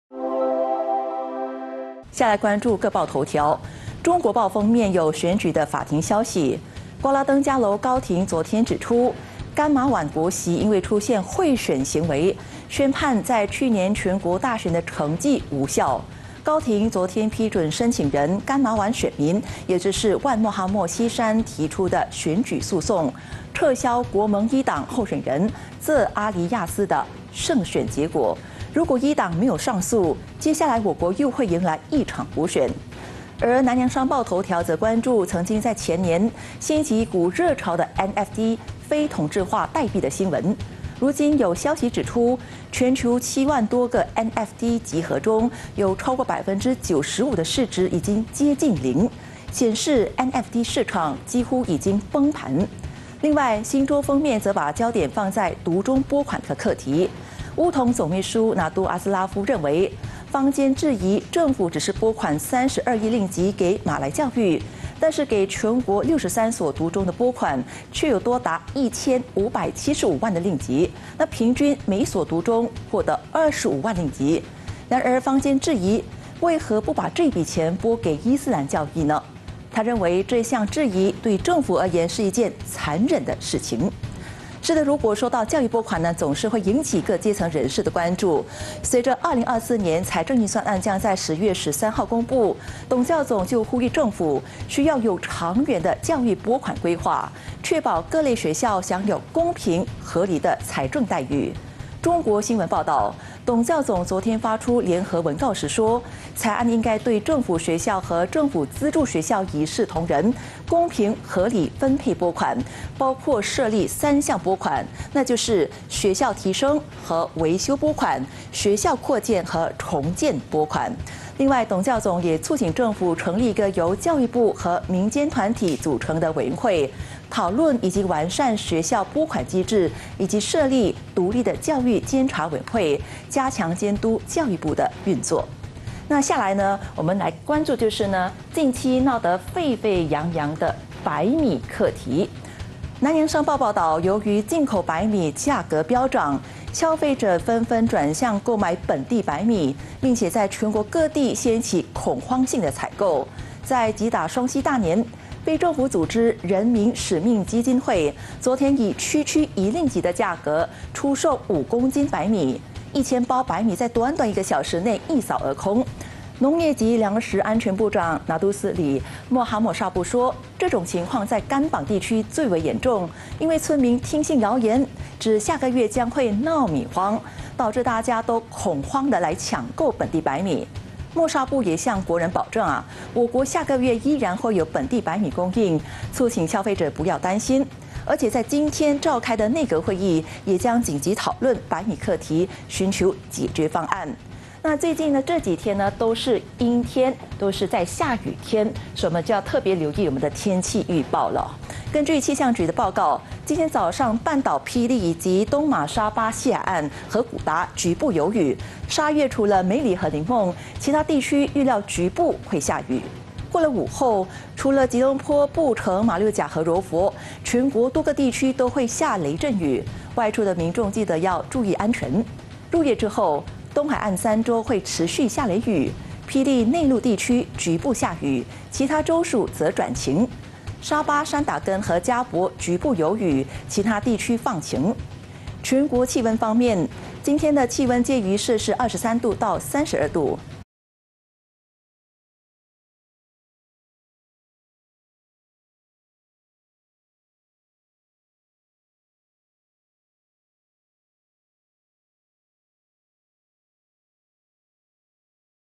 《天下速览》带您快速浏览各报头条【2023.09.27 八度早新闻】
八度早新闻《天下速览》带您快速浏览各报头条：
【早报焦点】
▪ 2024财案10月中公布  董教总:确保公平对待各校
▪ 听信缺货甘榜居民抢米  内阁今急商米荒解决问题
▪ 午后全马多地有雷阵雨  东海岸雨势持续至晚上